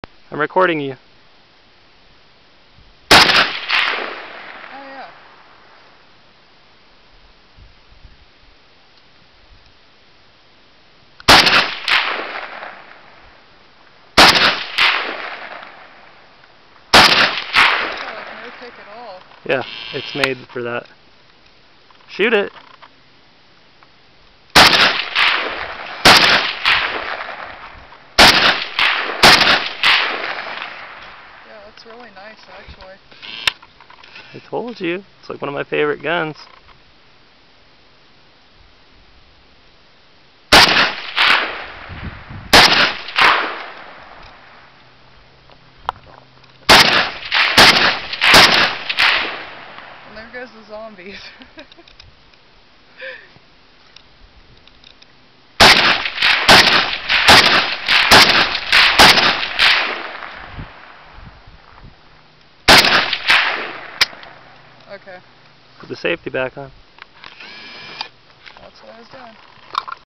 AK-47 without the ill-placed scope
Me standing on a bluff test firing my WASR-10 AK-47.

Please take a few seconds to rate my video after watching, thanks :)